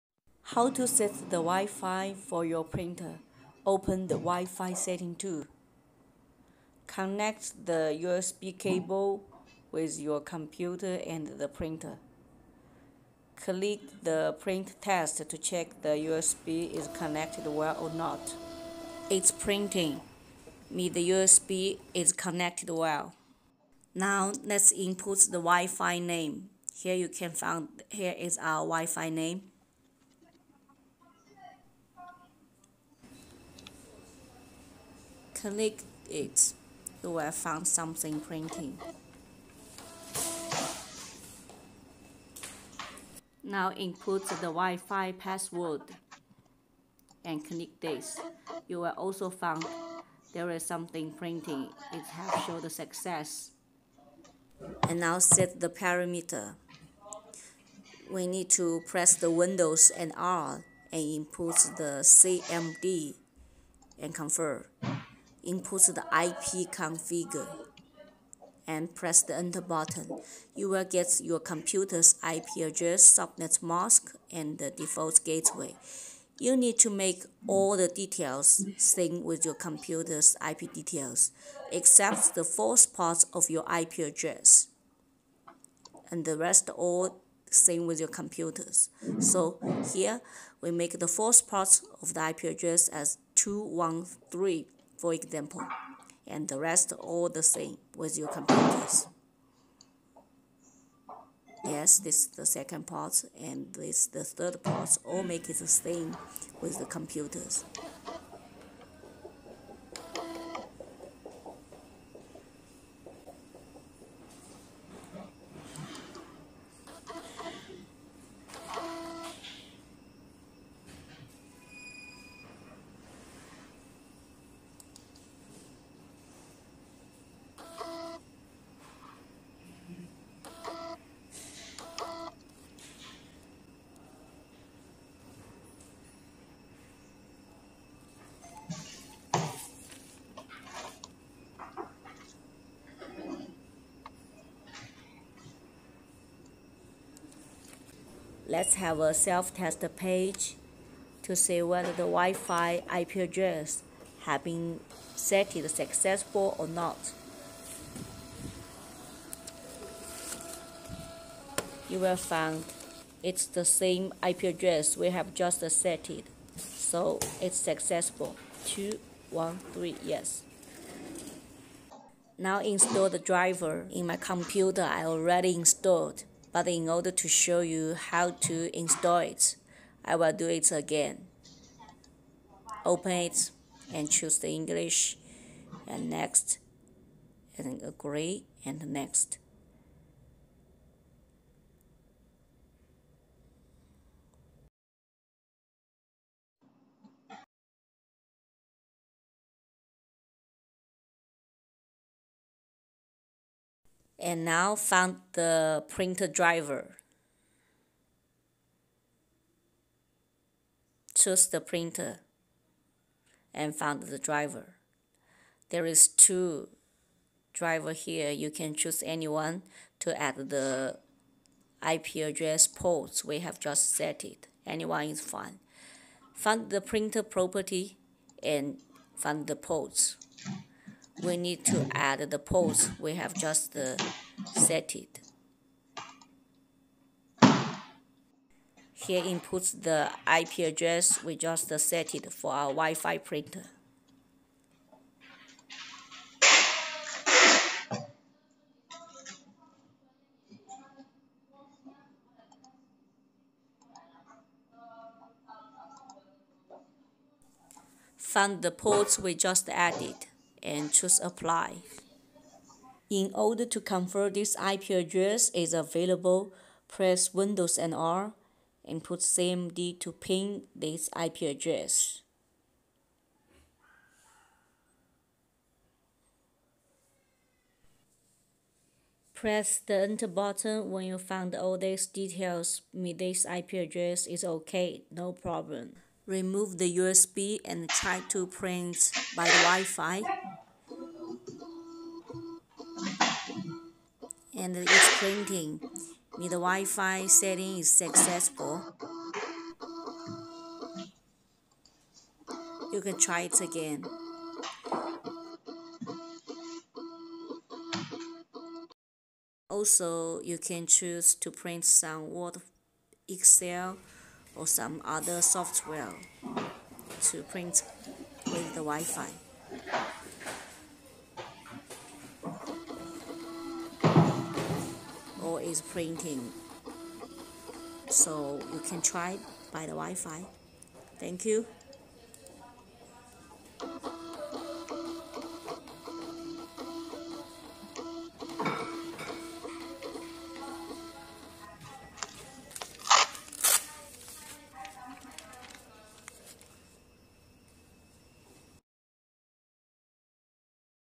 How to set WiFi IP address for your Hoin Printer. 100% let you know how to do.
Where you can get the Old WiFi Setting tool?
find "Old Version Wifi setting tool", download it. This is for printer with DIP at the back version, the old version printer sold before year 2020.